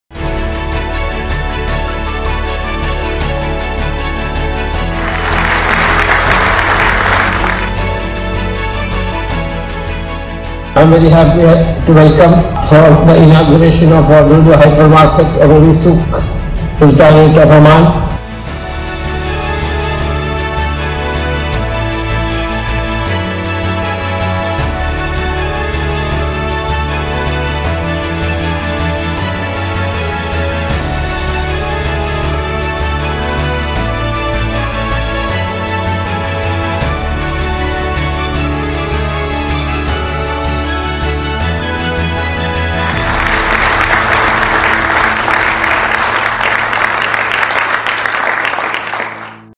LuLu Opens New Hypermarket in Ruwi, Oman
LuLu opened its newest store in Ruwi, the bustling commercial hub and the main business area of Muscat, located at the capital of Oman. The new store is the 27th branch in Oman, which has increased LuLu’s store count to 203 globally.